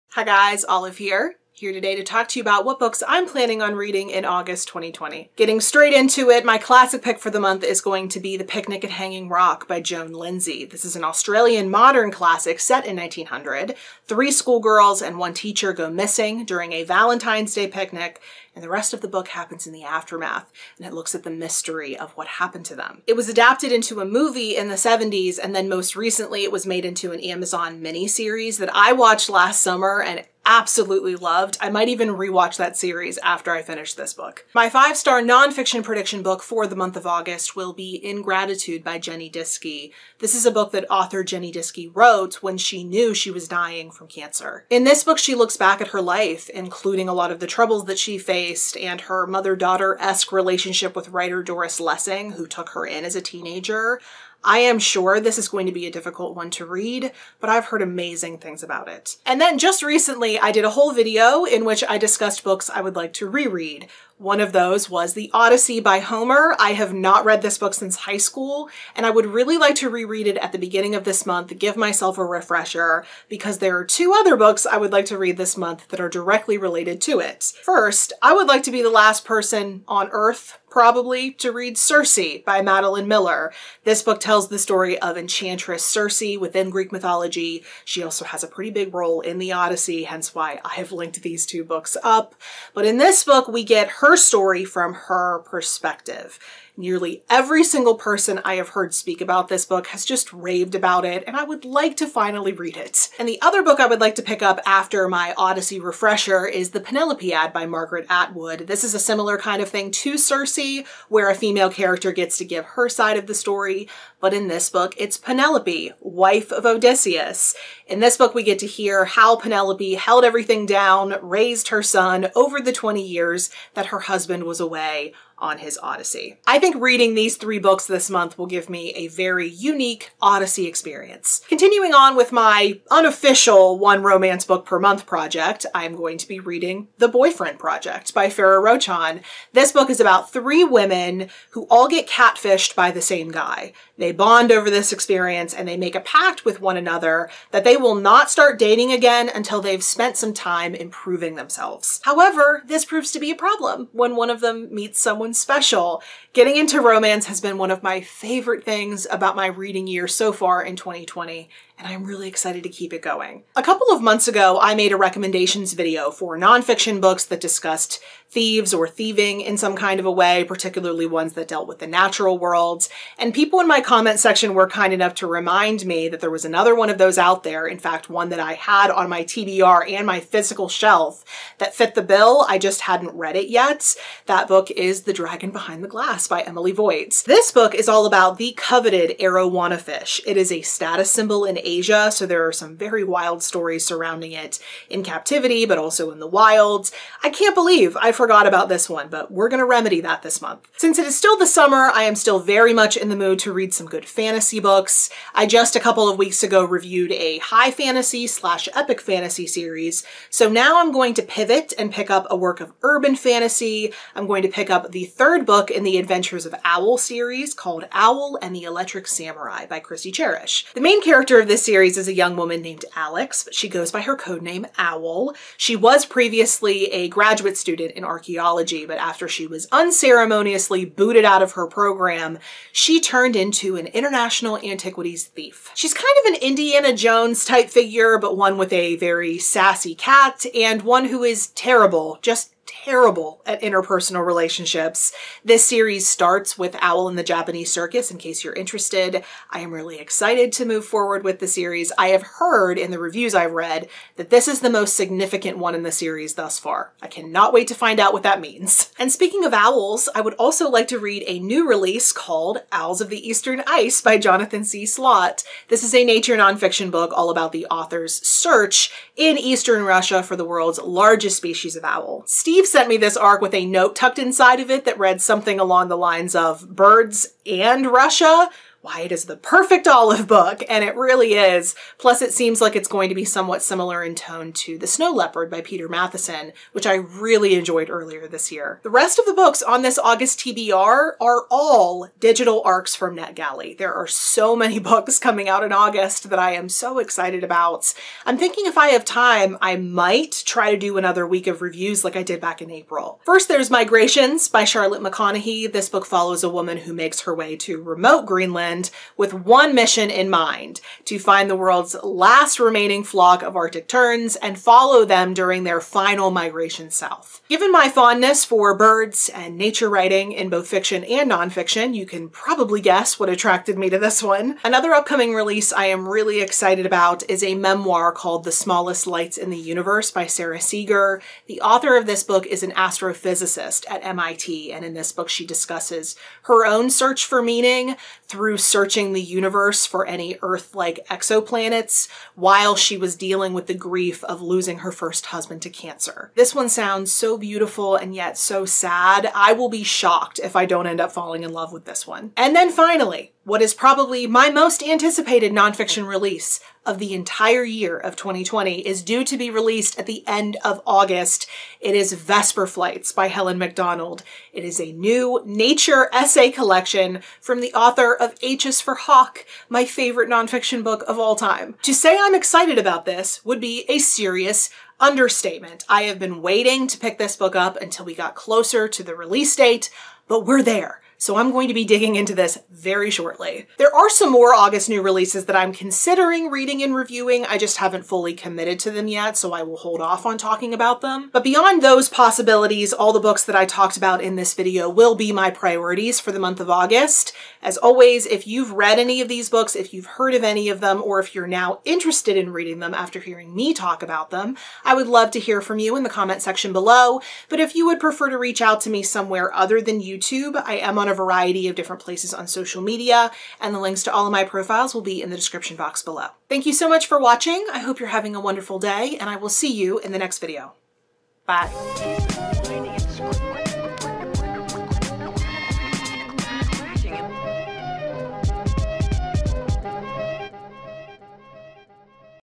August 2020 TBR
All the books I'm planning to read in August 2020!

--Books Mentioned--
Picnic at Hanging Rock by Joan Lindsay
In Gratitude by Jenny Diski
The Odyssey by Homer
Circe by Madeline Miller
The Penelopiad by Margaret Atwood
The Boyfriend Project by Farrah Rochan
The Dragon Behind the Glass by Emily Voight
Owl and the Japanese Circus by Kristi Charish
Owl and the Electric Samurai by Kristi Charish
Owls of the Eastern Ice by Jonathan C. Slaght
Migrations by Charlotte McConaghy
The Smallest Lights in the Universe by Sara Seager
H is for Hawk by Helen Macdonald
Vesper Flights by Helen Macdonald